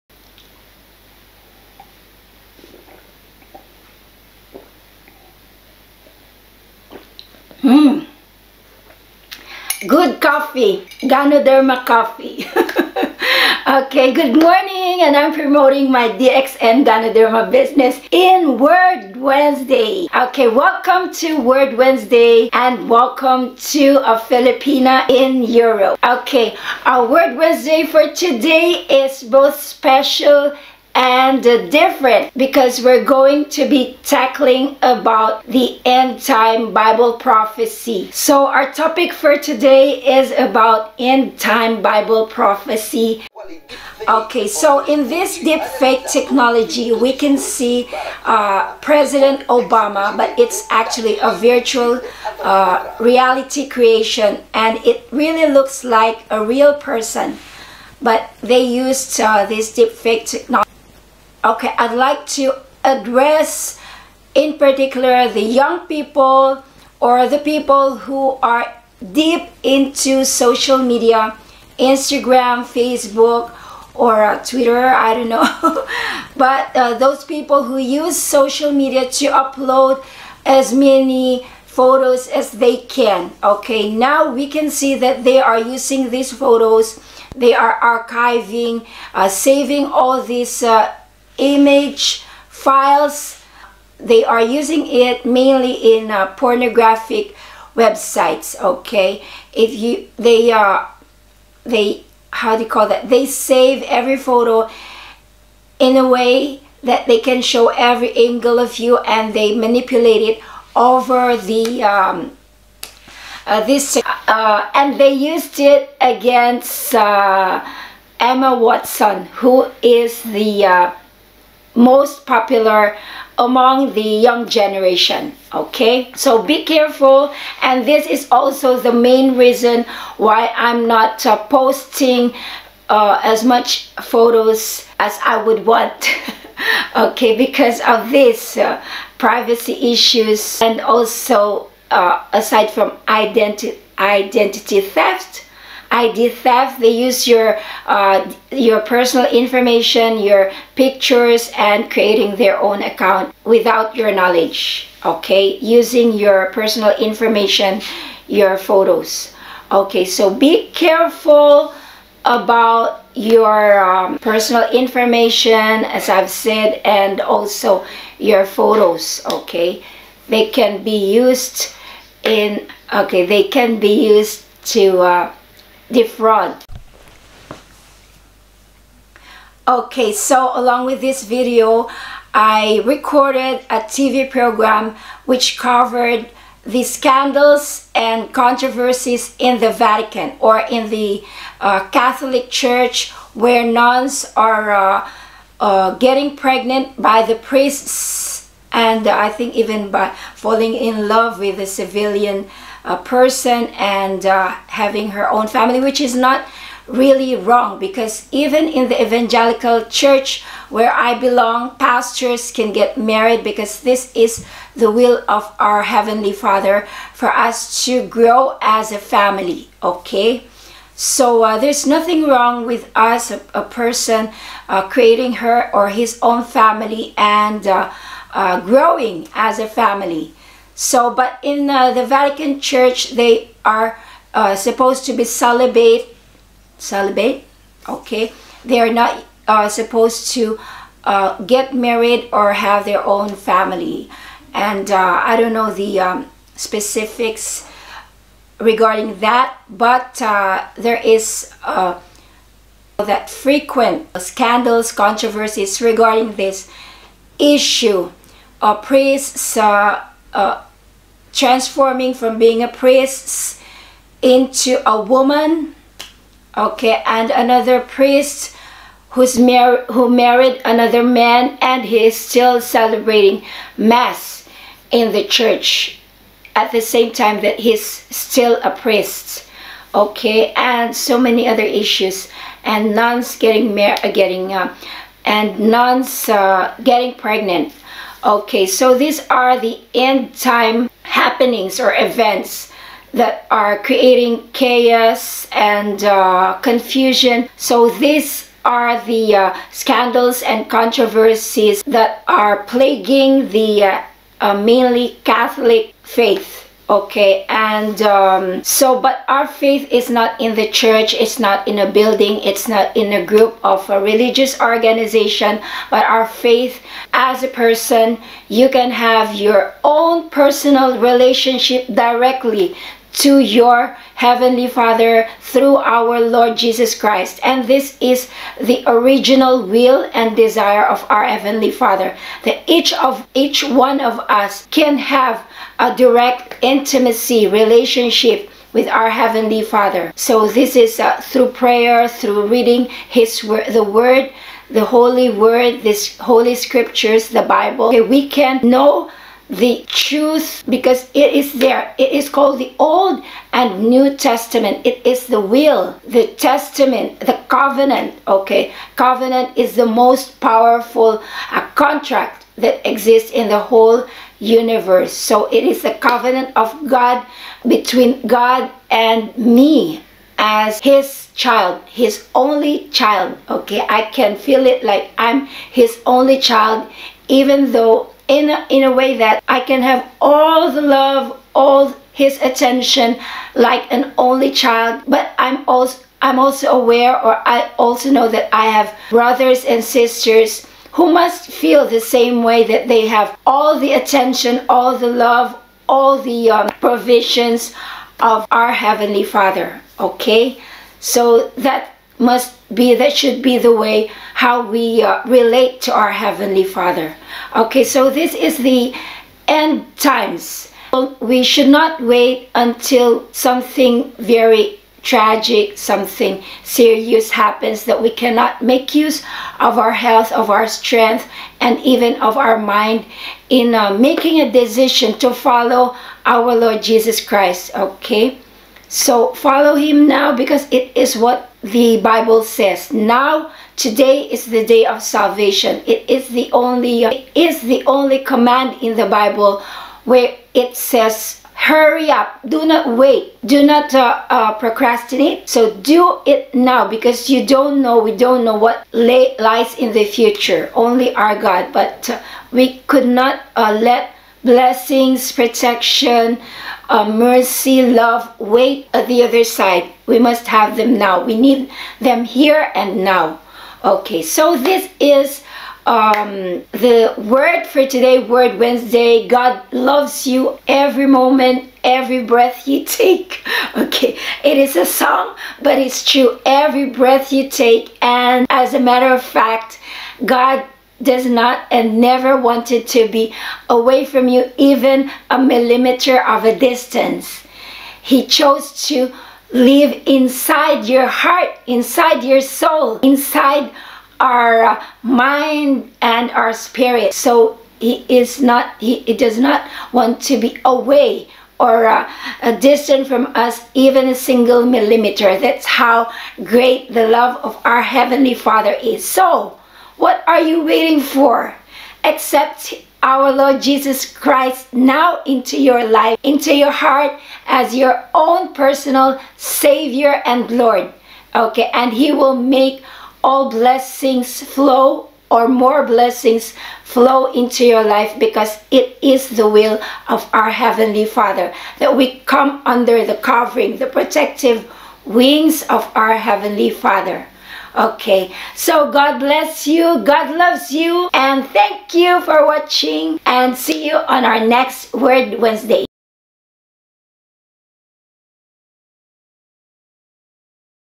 WORD Wednesday: What is "Deep Fake"?!
Good morning friends! I hope you're all doing great! Our message for today is about End-time Bible Propheciesand relative issues; our reference verses are:
 "The time will come when people will not put up with sound doctrine. Instead,to suit their own desires,they will gather around them a great number of teachers to say what their itching ears want to hear."
2Tim 4:3

"TODAY, if you hear HIS voice,do not harden your hearts!"
Hebrews 3:7